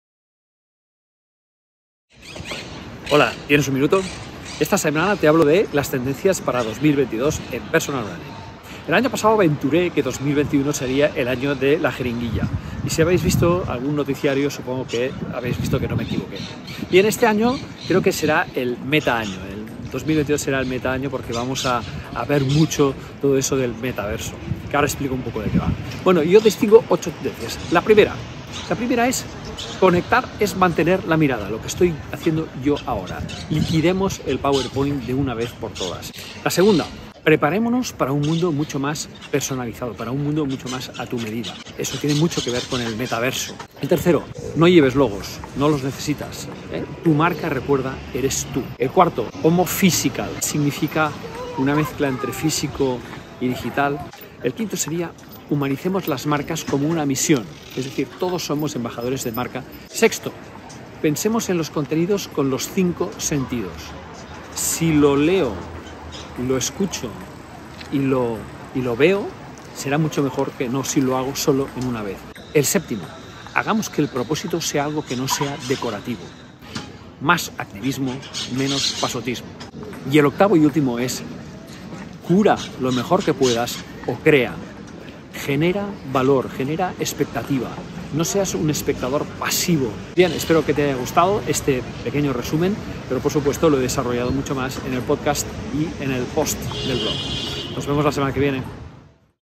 Tendencias 2022 en Personal Branding
Como los últimos años, expongo algunas de las tendencias que veo se van incorporando.

1. Liquidemos el PowerPoint: conectar es mantener la mirada
2. Preparémonos para el metaverso: el mundo hecho a tu exacta medida
3. No logo, no lo necesitas. Tu marca eres tú
4. Homo Phigital, menos pantalla, más carne, toca equilibrar
5. Humanizar marcas como misión, todos somos embajadores
6. Contenidos en los 5 sentidos: si podemos leer, escuchar y mirar, mejor
7. El propósito no es decorativo. Más activismo, menos pasotismo
8. Cura lo mejor que puedas o crea, genera expectativa y valor